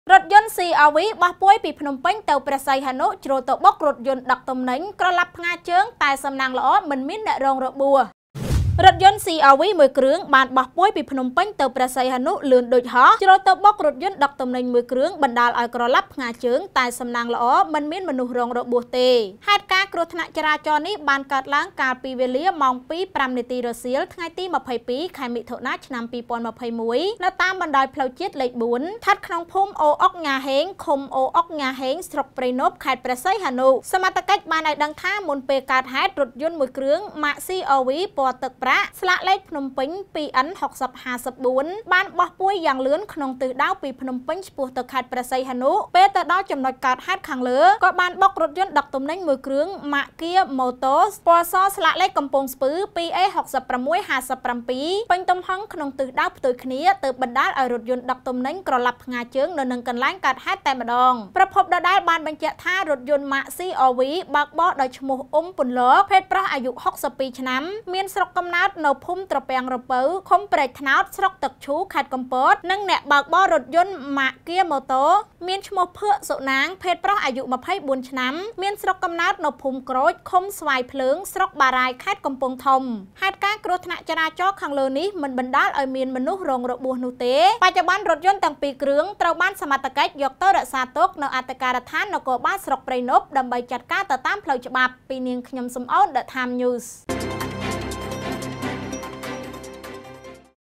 រថយន្តCRV បោះពួយយ៉ាងលឿនពីភ្នំពេញ-ព្រះសីហនុ ជ្រុលទៅបុ..ករថយន្តដឹកទំនិញក្រឡាប់ផ្ញារជើង
ទឹកដោះម្ដាយថ្លៃ! រថយន្តCRV បោះពួយយ៉ាងលឿនពីភ្នំពេញ-ព្រះសីហនុ ជ្រុលទៅបុ..ករថយន្តដឹកទំនិញក្រឡាប់ផ្ញារជើង សំណាងល្អមិនមានអ្នករងរបួស

លោកអ្នក អាចទាក់ទងផ្ដល់ព័ត៌មានទាន់ហេតុការណ៍ 24/7 មកកាន់សារព័ត៌មាន DE TIME NEWS តាមរយៈទូរសព្ទលេខ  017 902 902